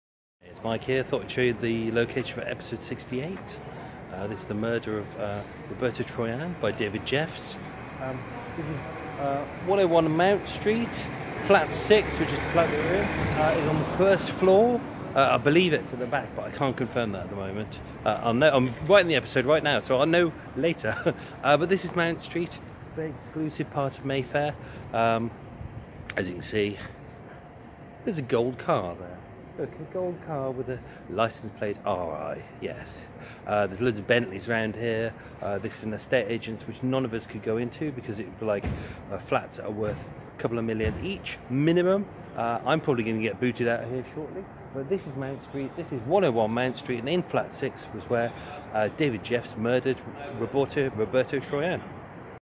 Ep68 - Roberto Troyan - Grief, Guilt and Greed - Murder Mile True-Crime Podcast (location video)
Ep68: Roberto Troyan - Grief, Guilt and Greed. On Friday 8th March 2013, wealthy Mayfair socialite Roberto Troyan; a gregarious, fun and generous man who being gripped with grief and unable to cope placed his life in the hands of someone he felt he could trust - his financial advisor - and yet this greedy little man would bleed Roberto dry.

Date: Friday 8th March 2013
Location: Flat 6, 101 Mount Street, Mayfair, W1
Victims: 1 (Roberto Charles Troyan)
Culprit: David Jeffs

This is the location of 101 Mount Street where Roberto Troyan lived and died.